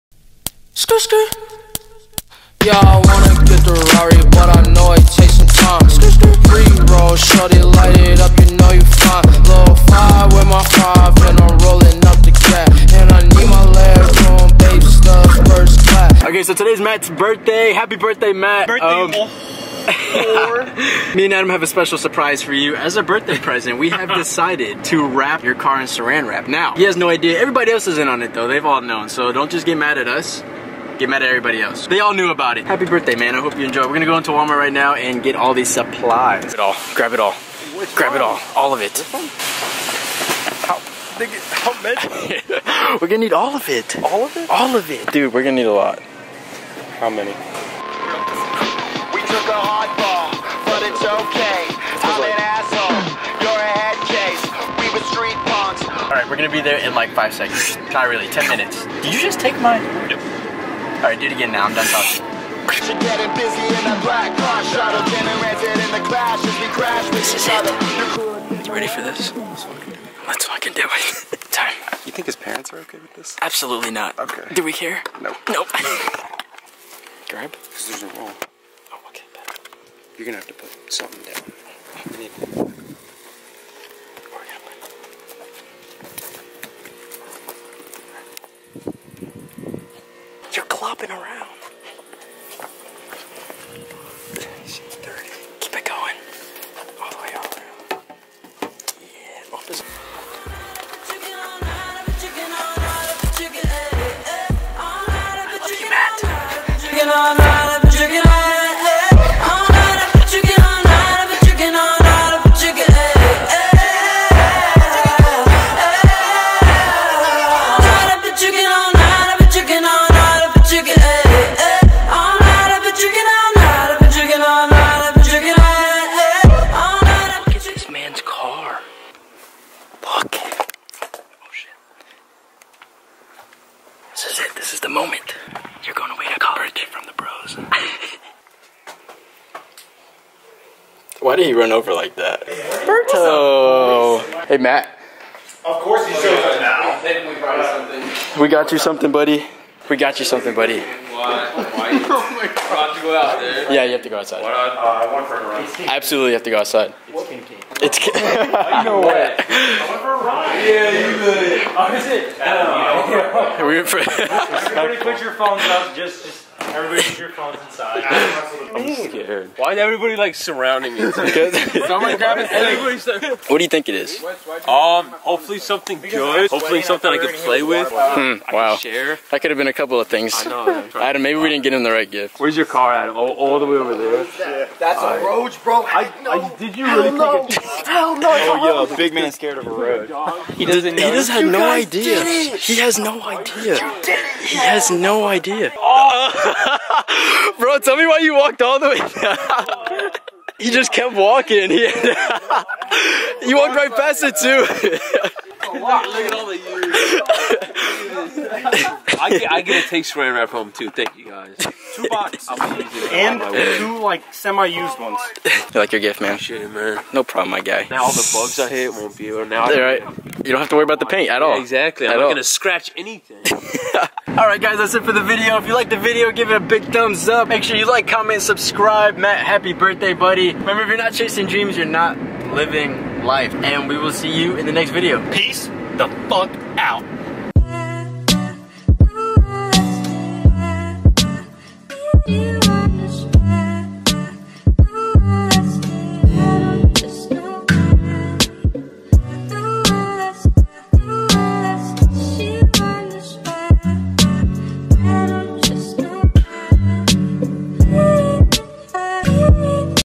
HILARIOUS SARAN WRAP CAR PRANK!!
Adam I pick the perfect Birthday/Going Away present for Matt.
Watch the best SARAN WRAP CAR PRANK & hope you Enjoy the video.

                                           ^^^ CLICK HERE ^^^